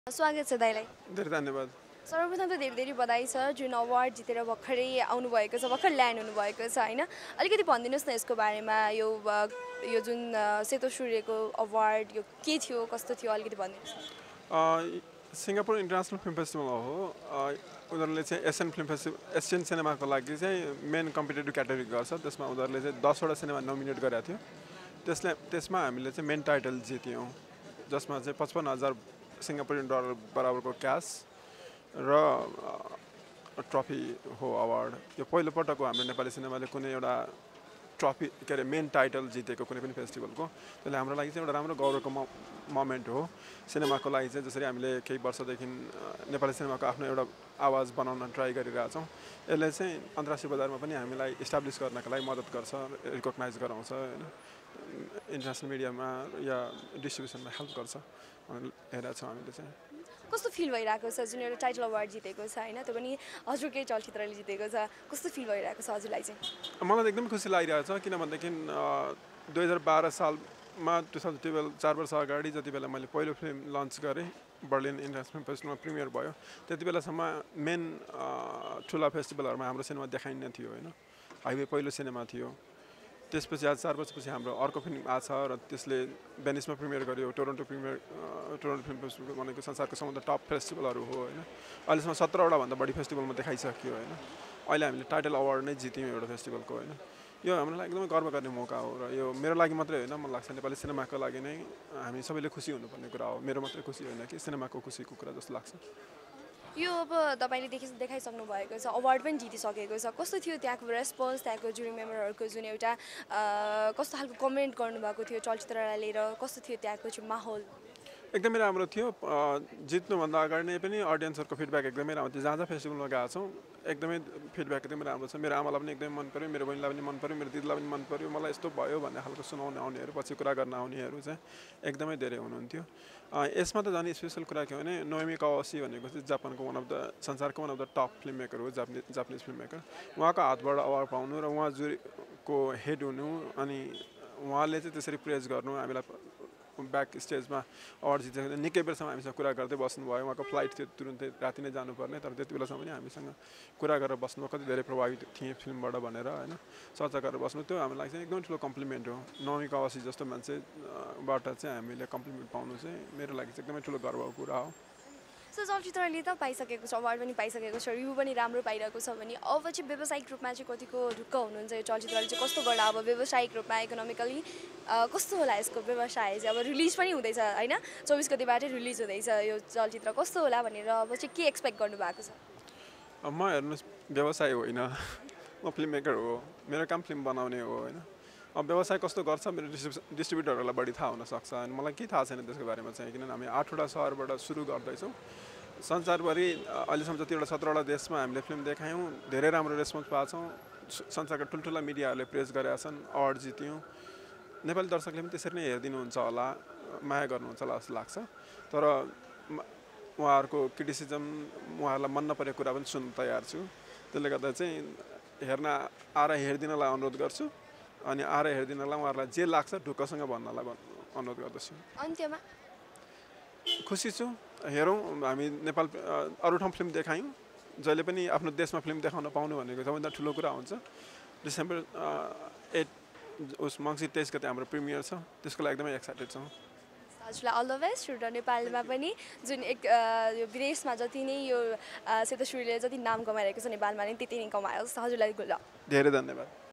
सेतो सूर्यका निर्देशक दीपक रौनियारसँगको कुराकानी | Medianp.com
Medianp Presents to you an exclusive interview with director Dipak Rauniyar.

The director has just returned from Singapore from an event. There his movie 'Seto Surya' won and he received a trophy and 55 thousand worth of  Singapore Dollar. This is the first time a  Nepali movie has won an international main title.